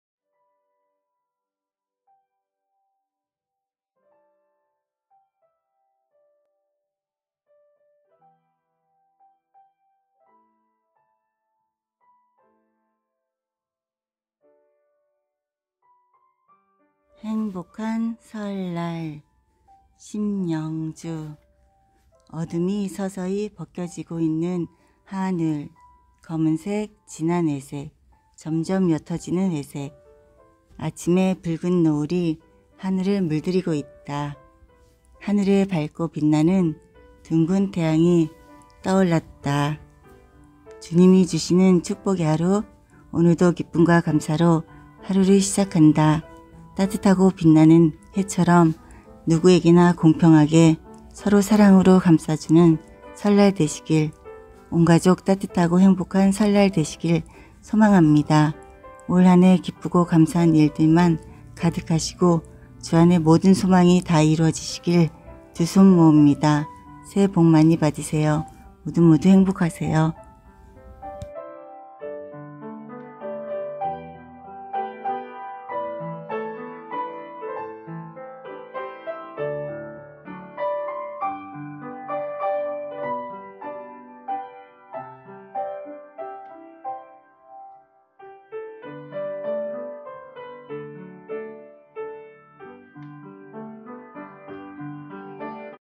행복한 설날(성시, 아침묵상)하나님의 마음 심영주
🌅행복한 설날

              심영주

어둠이 서서히
벗겨 지고 있는 하늘.

검은 색, 진한 회색.
점점 옅어 지는 회색.

아침의 붉은 노을이
하늘을 물들이고 있다.

하늘에 밝고 빛나는
둥근 태양이 떠올랐다

주님이 주시는
축복의 하루.

오늘도 기쁨과 감사로
하루를 시작한다.

따뜻하고 빛나는 해처럼
누구에게나 공평하게

서로 사랑으로
감싸 주는 설날 되시길...

온 가족 따뜻하고
행복한 설날 되시길
소망합니다.

올 한해 기쁘고 감사한
일들만 가득하시고

주 안에 모든 소망이
다 이루어 지시길
두 손 모읍니다.

모두 모두 행복하세요.